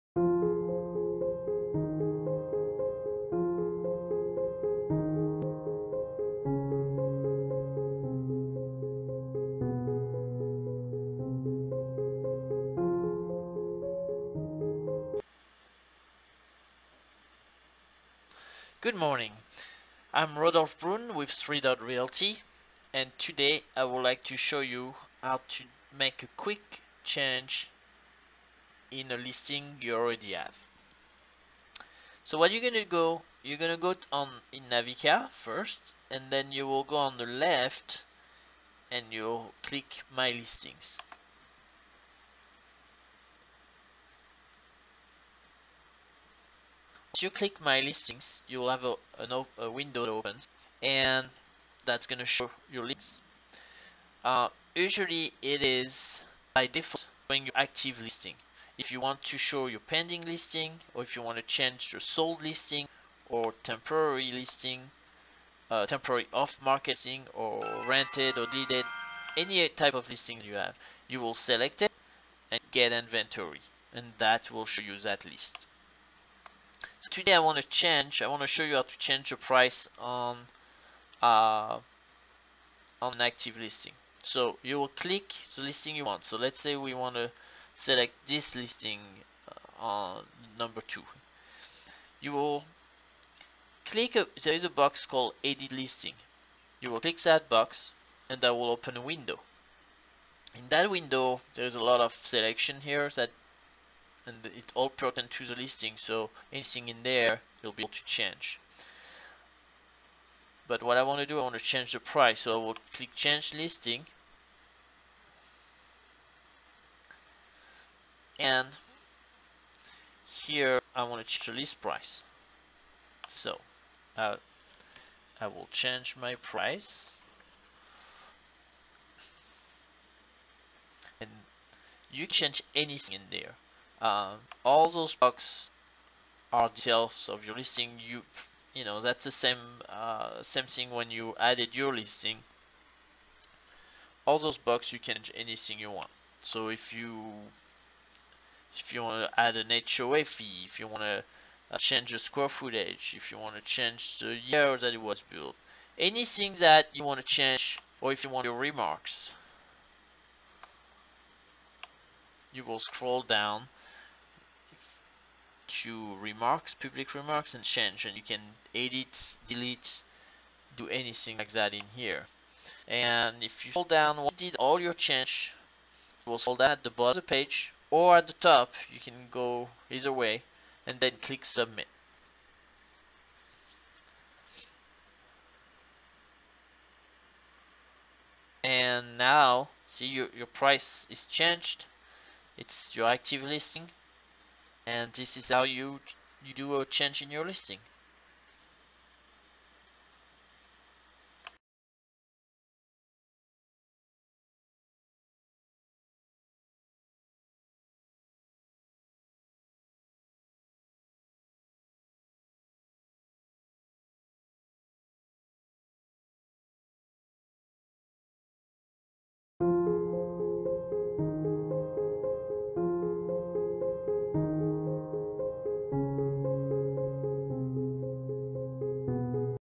Tutorial Video Making a change to a listing using Navica
How to make a change to one of your listing on Navica MLS? Tutorial help video by 3 DOT Realty, LLC